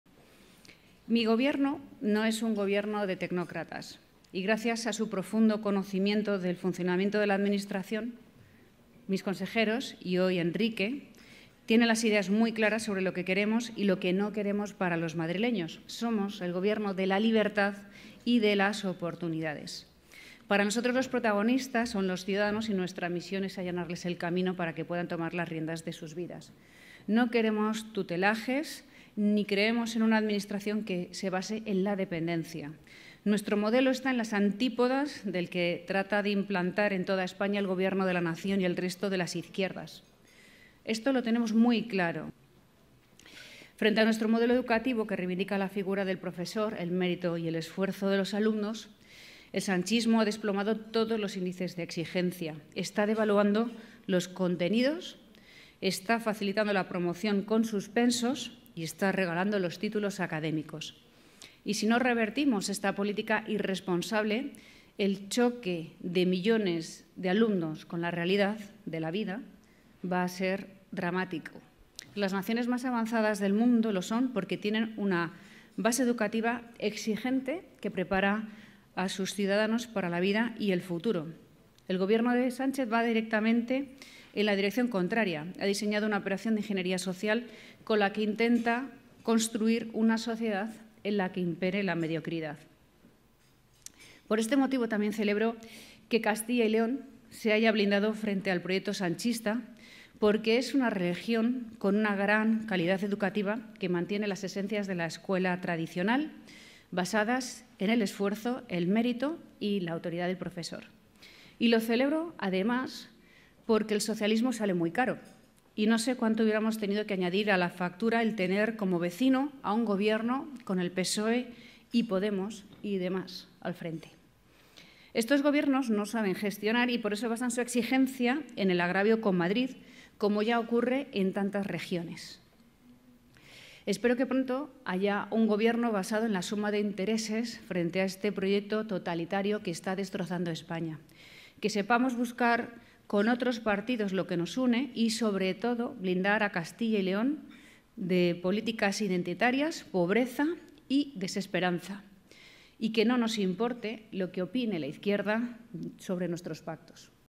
AYUSO tiende la mano a ABASCAL: “¡Que no nos importe lo que opine la izquierda de nuestros pactos!”
La presidenta de la Comunidad de Madrid, Isabel Díaz Ayuso, ha celebrado que Castilla y León, en las elecciones del 13F, se haya "blindado frente al proyecto sanchista".

"El socialismo sale muy caro", ha resaltado. Además ha tendido la mano a VOX.